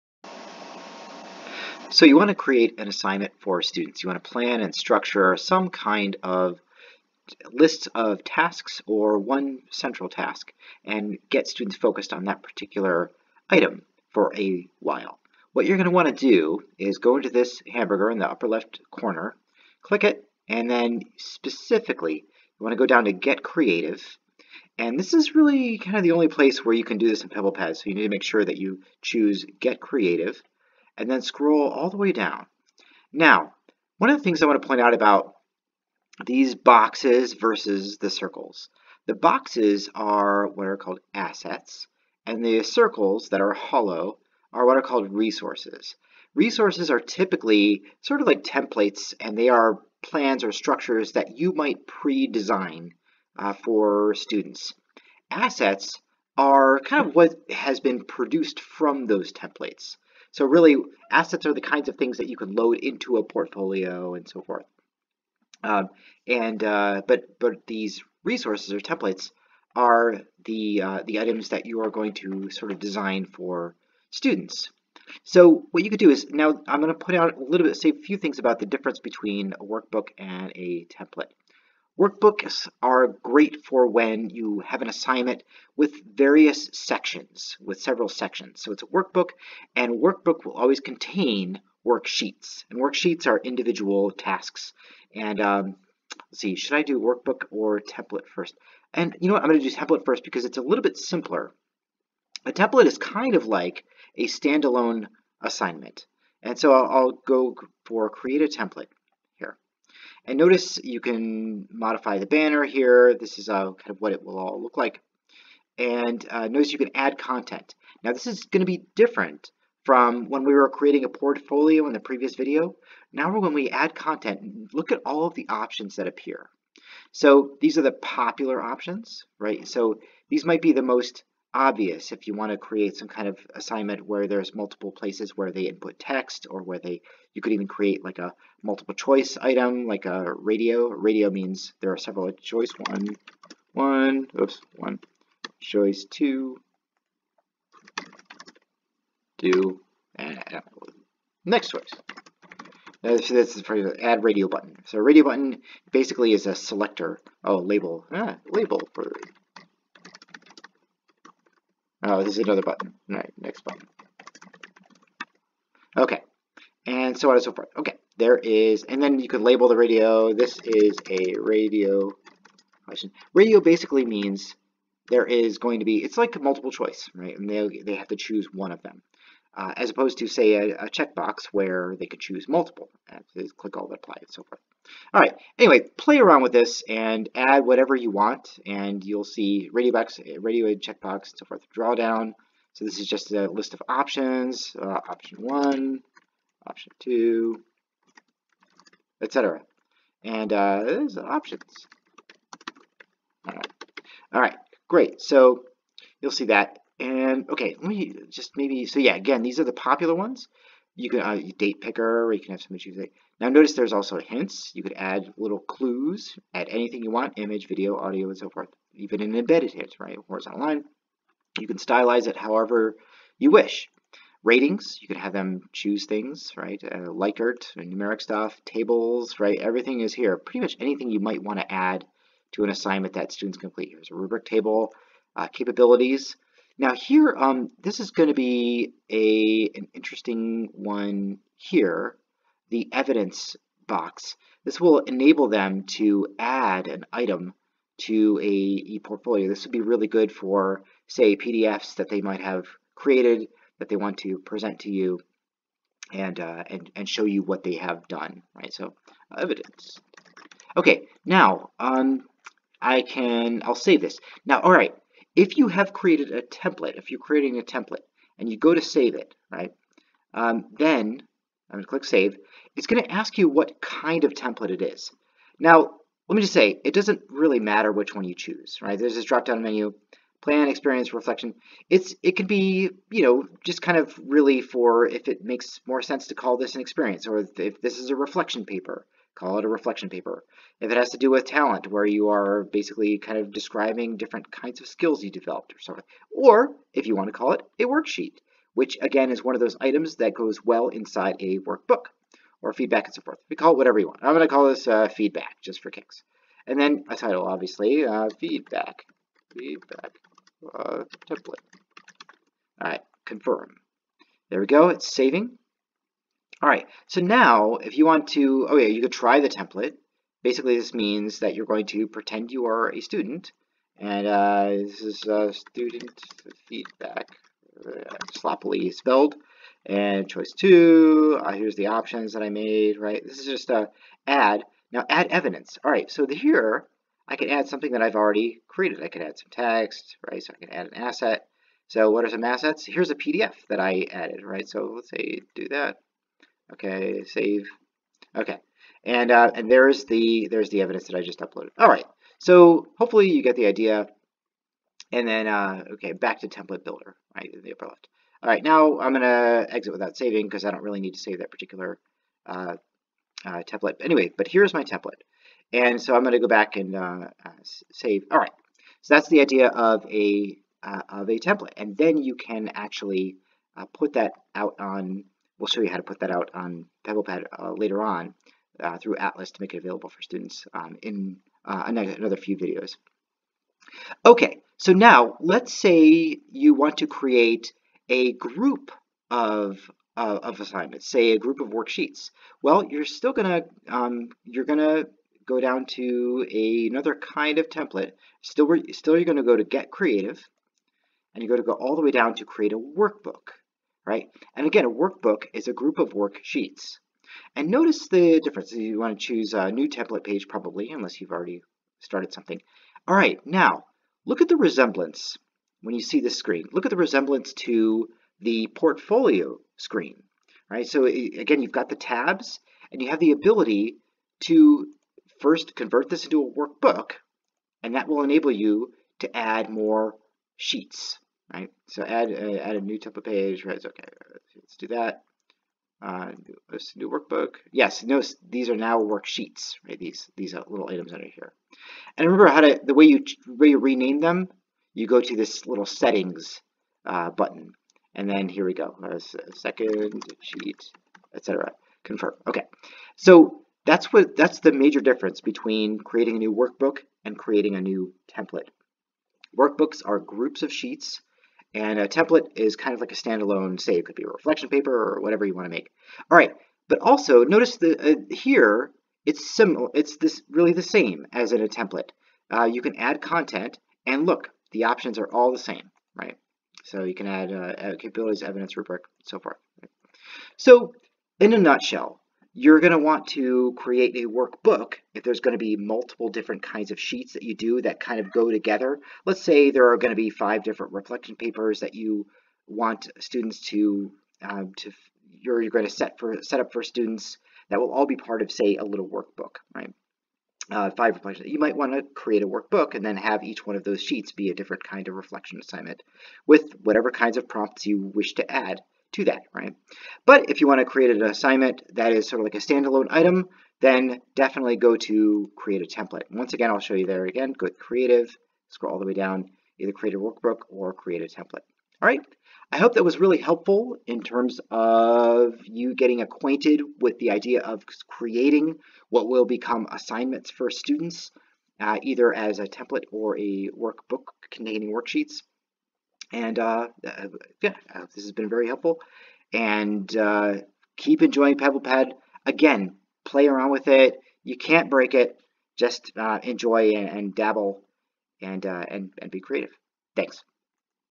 PebblePad | Creating Templates and Workbooks (Resources)
When making assignments, how to
1. create a Template* and
2. create a Workbook. (A workbook is a group of worksheets.)
*A template can be categorized as a Plan, Experience, Reflection, Talent, Worksheet, or Feedback. The category you select does not affect the template at all; it is only for the purpose of labelling.